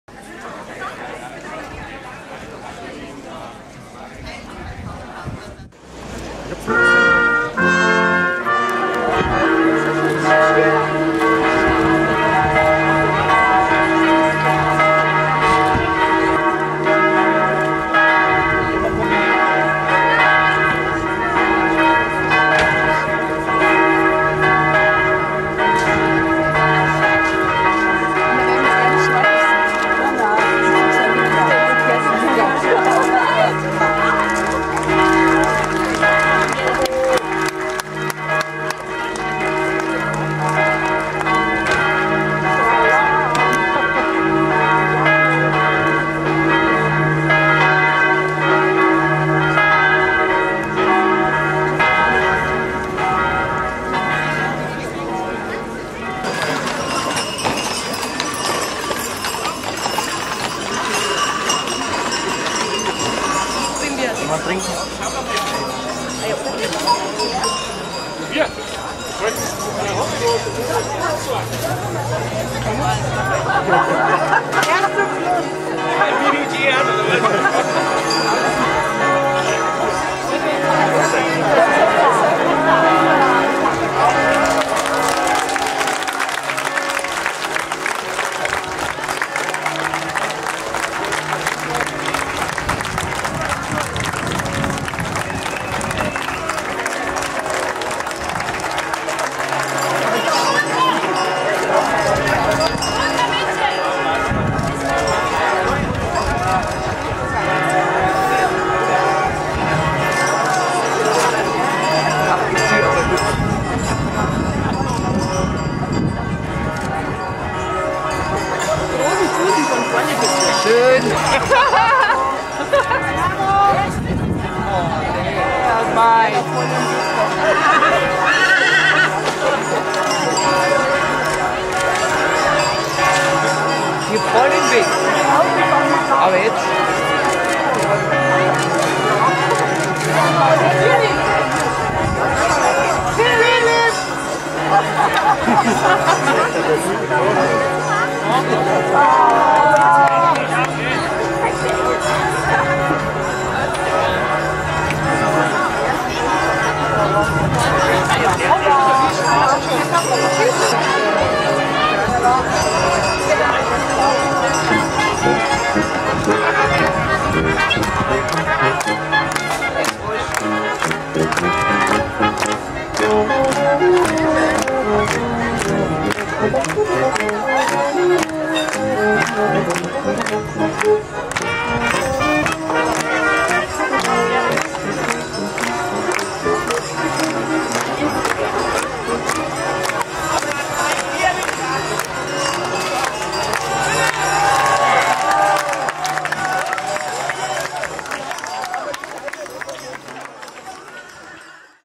Philipp Lahm und Claudia - Hochzeit - Wedding - Filmproduction Munich
Kapitän Lahm und Claudia heirateten am 14. Juli 2010 in Aying. Die kirchliche Zeremonie fand in der kleinen und romantischen Kirche von Kleinhelfendorf statt. Bei herrlichen Wetter und bayerischer Stimmung. Hier ein Video von Frederik Hettich
Filmproduktion München, Aying Ottobrunn Brunnthal Videoproduktion Höhenkirchen München, Imagefilm München,